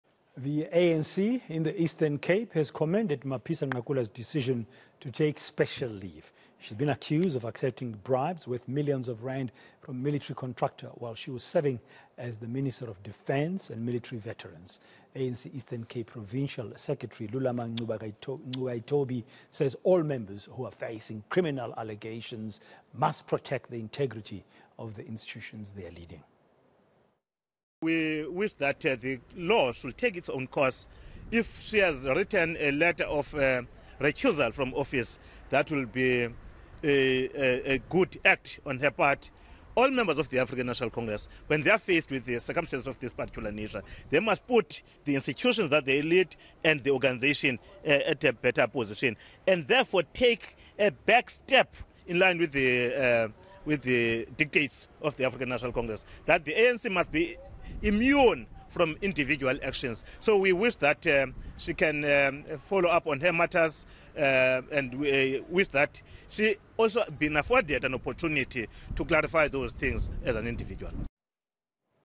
'ANC must be immune to individual's actions'
The ANC in the Eastern Cape has commended Nosiviwe Mapisa-Nqakula's decision to take special leave.